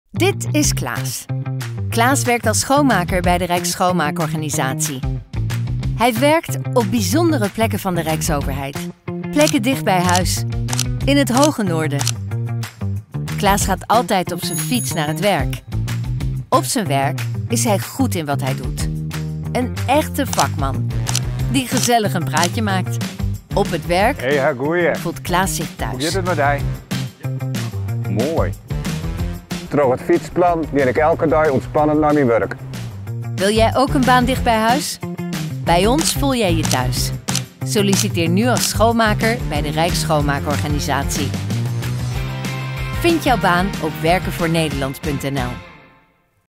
Dit is Klaas!
Klaas werkt als schoonmaker bij de Rijksschoonmaakorganisatie. Hij werkt op bijzondere plekken van de Rijksoverheid in het hoge Noorden. Plekken dichtbij huis: hij fietst maar een paar minuutjes naar zijn werk. Op zijn werk is hij goed in wat hij doet, een echte vakman die gezellig een praatje maakt. Op het werk voelt Klaas zich thuis.

Wil jij ook een baan dichtbij huis?

Bij ons voel jij je thuis! Solliciteer nu als schoonmaker bij de Rijksschoonmaakorganisatie. Vind jouw baan op werkenvoornederland.nl